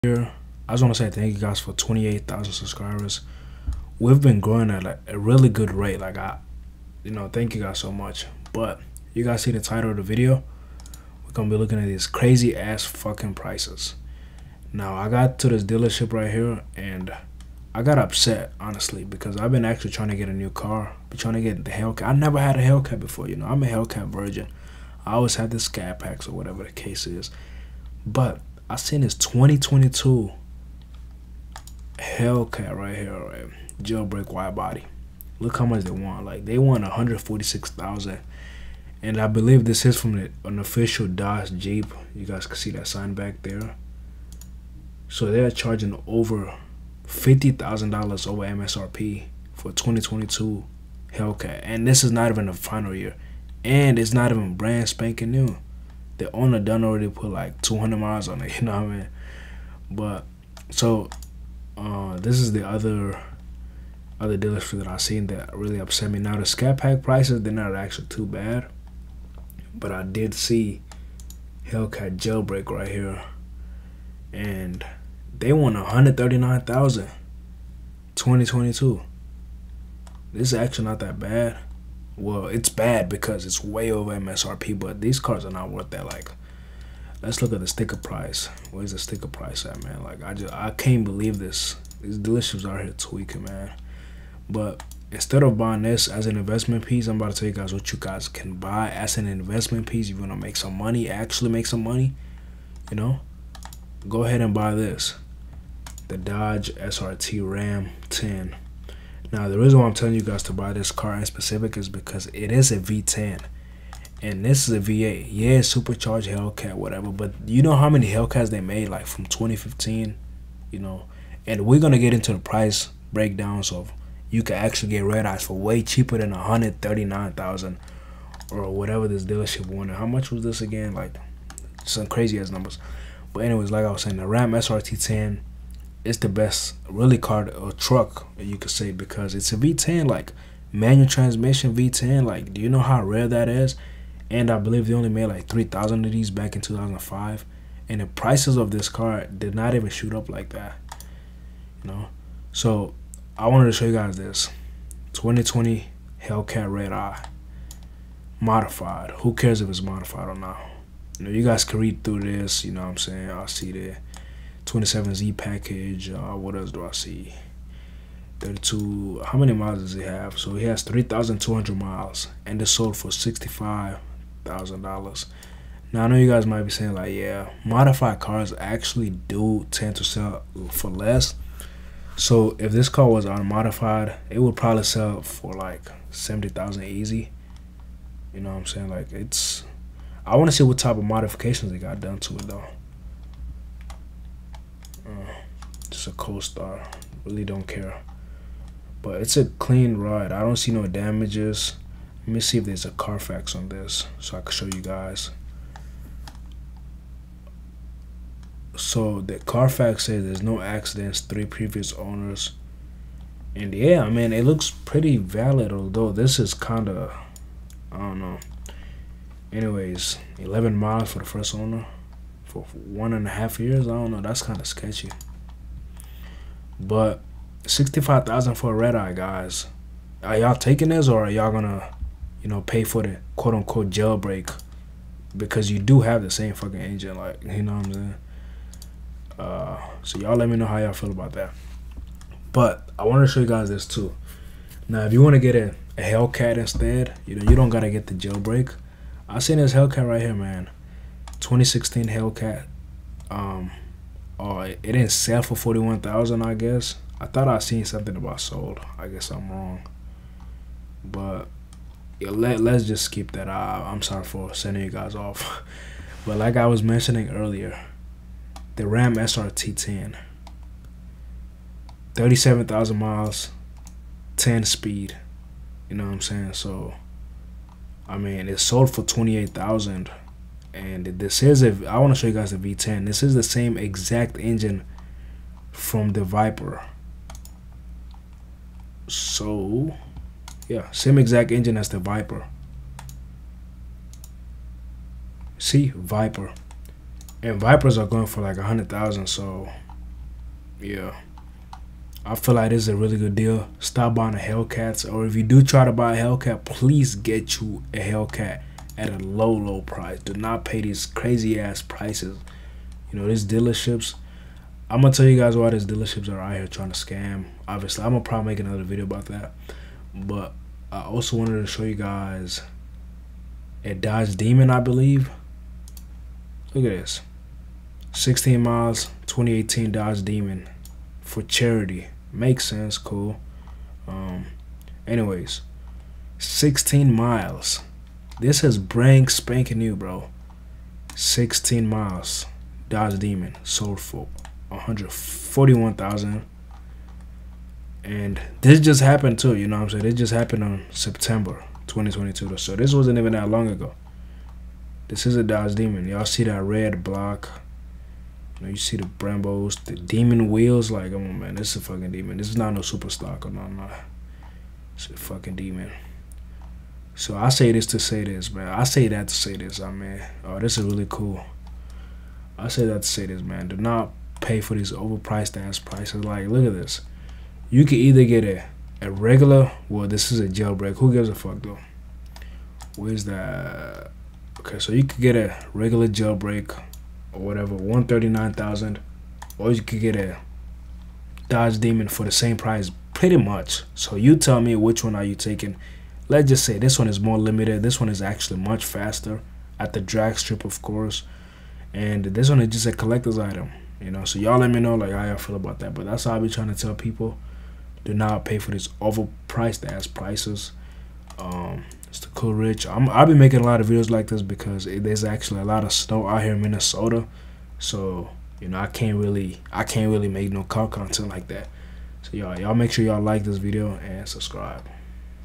DELUSIONAL Dealership Wants $150,000 For A USED Hellcat..😂
MOST DEALERS Will want to make u Pay to Play, well go over crazy Dealers asking outrageous prices for hellcats and jailbreak redeye hellcats! i also show comparison on the prices with the most recent sales of the Dodge Ram Srt10 pickup truck and the Dodge Demon.
Make sure you guys like & sub to join the Cool Fam.